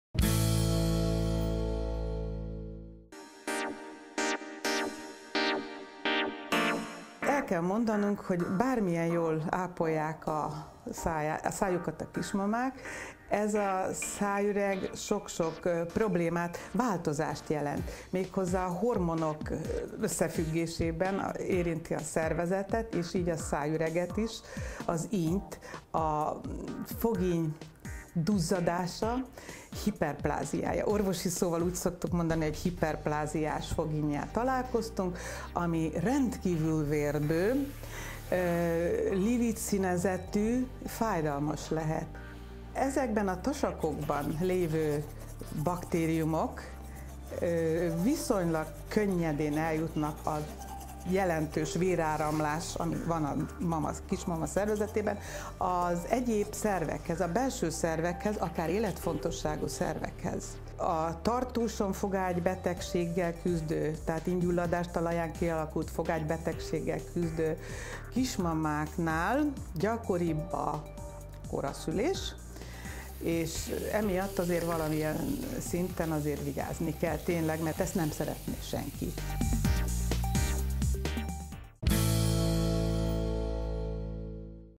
Koraszüléshez vezethet a tartós fogínygyulladás?
Dr. Dima Mária Magdolna fogszakorvos tanácsai kismamáknak...